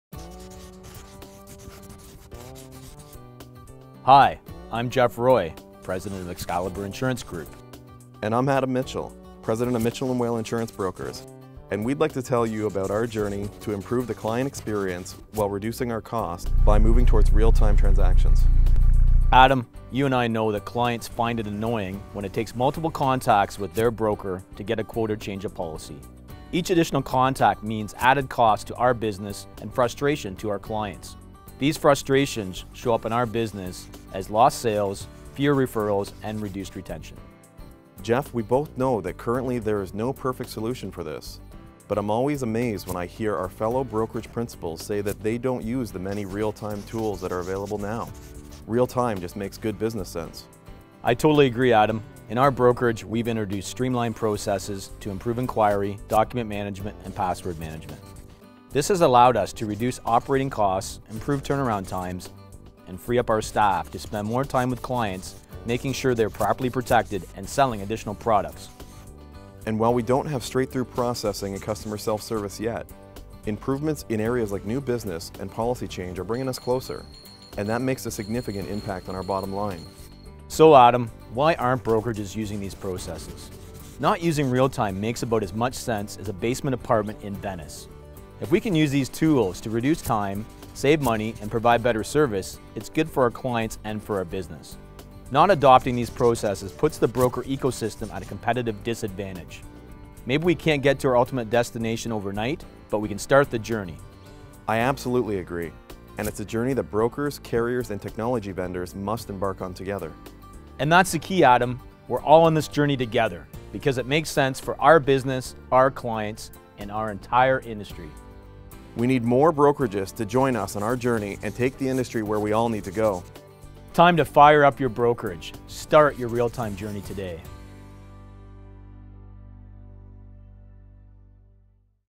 The Dollars and Sense Behind the Journey to Real Time
Increasing the efficiency of your insurance brokerage makes good business sense.

Canadian P&C brokers Jeff Roy and Adam Mitchell share their journey to make gains in efficiency, profitability and customer service by adopting Real Time processes.

Although there currently is no perfect solution, there is a lot that can be done today to produce meaningful results.

Join Jeff and Adam as they describe their journey to Real Time.